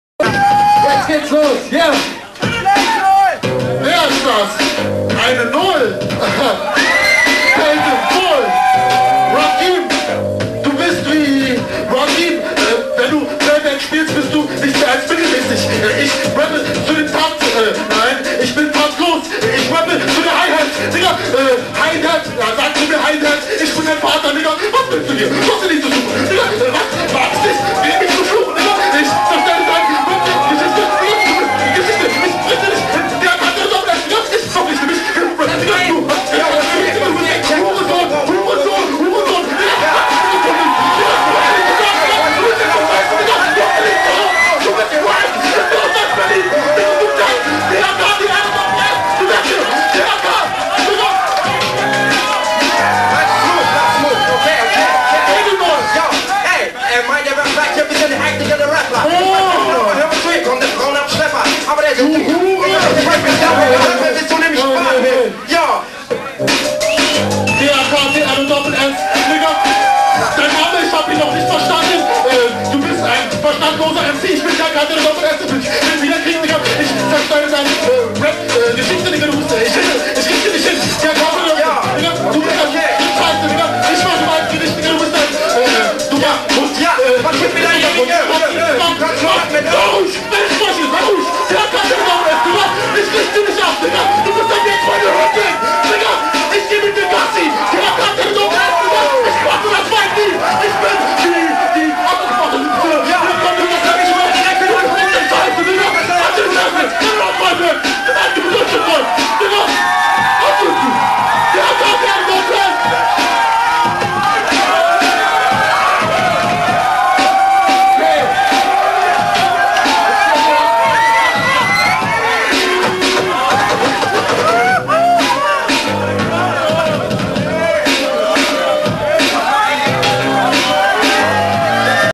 Taktloss rastet aus (Freestyle Battle)
Taktloss rastet beim battlen aus und macht seinen gegner fertig